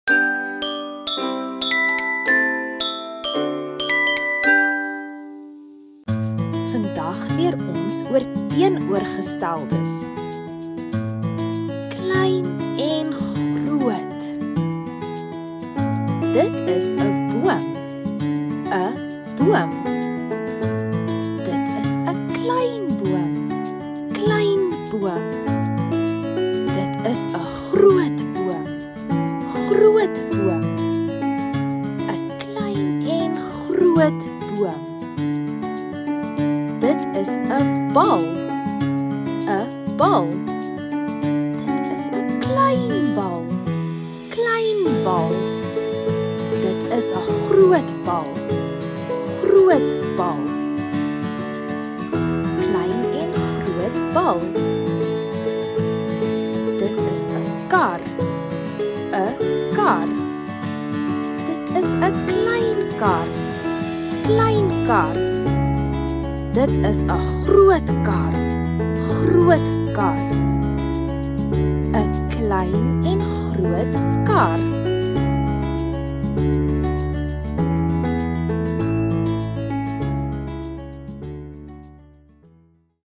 Afrikaans - Teenoorgesteldes - Klein en Groot
Ons klein maatjies leer van teenoorgesteldes - 'klein' en 'groot' om hulle woordeskat te verbeter. Hierdie video is vir ons klein maatjies wat begin praat; en help veral ons twee-jarige maatjies om nuwe woorde en twee/drie-woord sinne te sê.